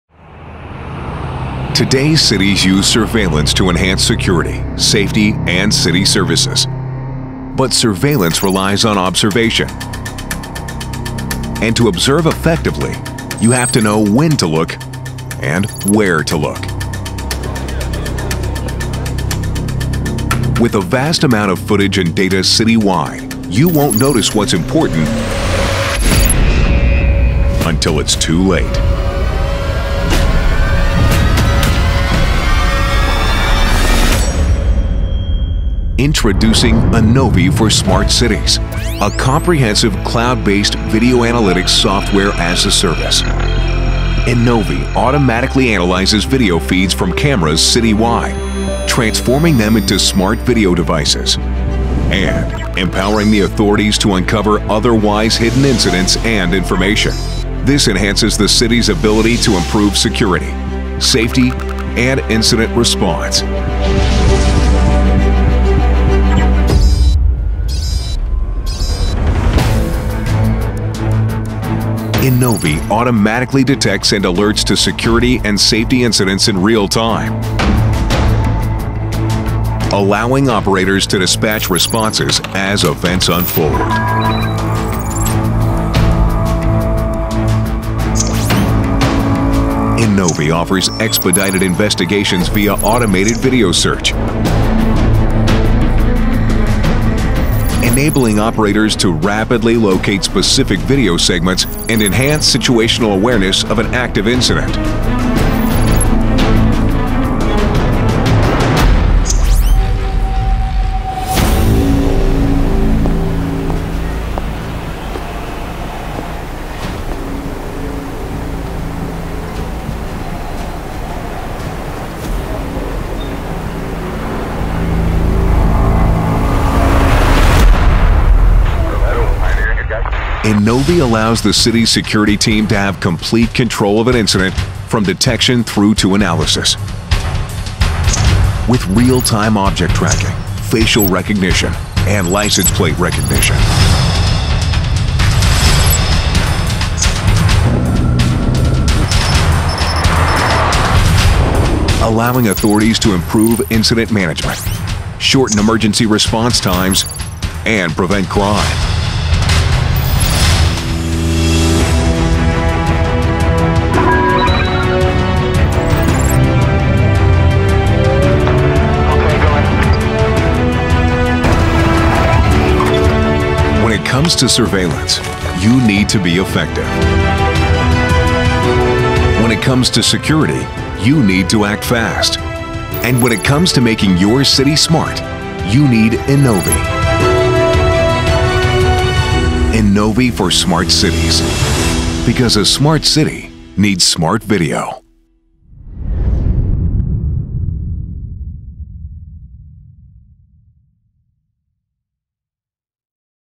innoVi for Smart Cities
innoVi for Smart Cities, by Agent Video Intelligence (Agent Vi), is a cloud-based, innovative video analytics Software-as-a-Service (SaaS). It transforms the hundreds or even thousands of surveillance cameras deployed across the city into smart video devices contributing to the city's ability to improve security, safety and incident response city-wide. innoVi empowers authorities to uncover otherwise hidden incidents, insights and information, and allows true leveraging of the municipal surveillance infrastructure, even in the most dynamic and challenging urban environment.